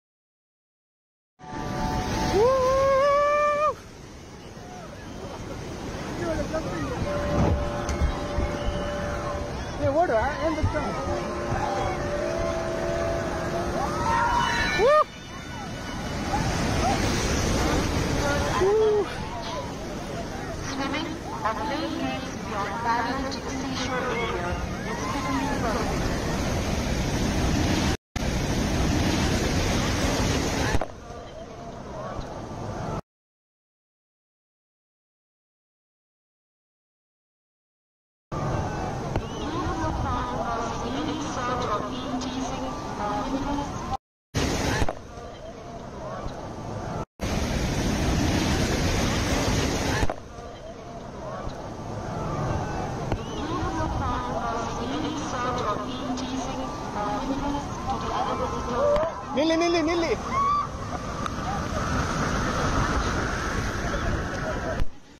Discover the Likoni Floating Bridge Latest Updates & In-Depth Overview
"Explore the engineering marvel of the Likoni Floating Bridge! In this video, we bring you the latest updates, detailed insights, and fascinating facts about this incredible infrastructure project. Learn about its construction, impact on the community, and future developments.